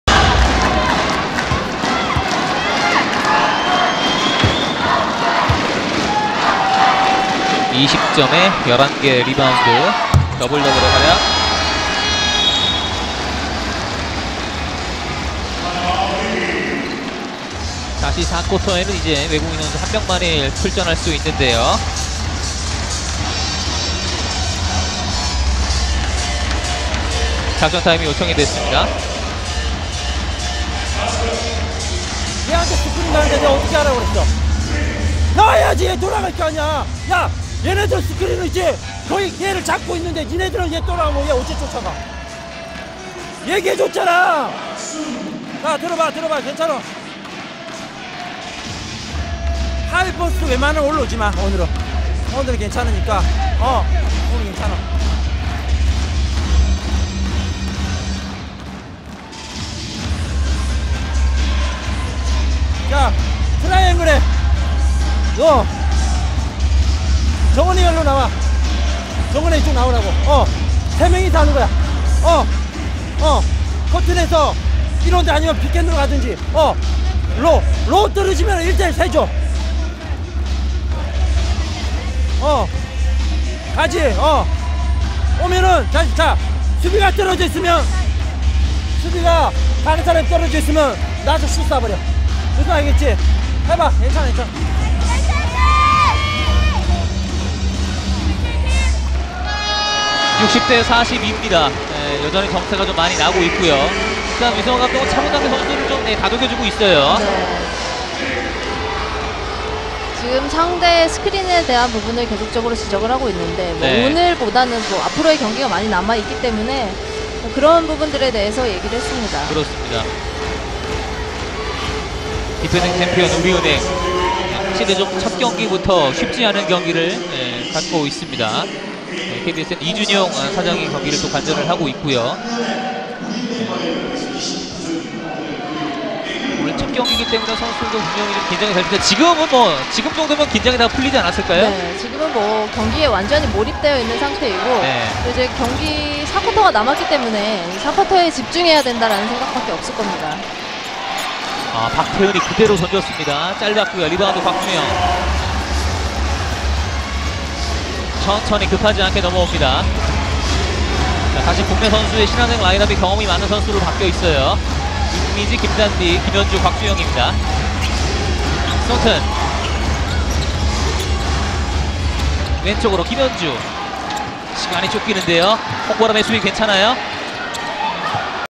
2017 WKBL 여자 프로농구 - 우리은행의 작전타임! 새로운 작전을 지시하는 위성우 감독
2017 WKBL 여자 프로농구 - 우리은행의 작전타임! 새로운 작전을 지시하는 위성우 감독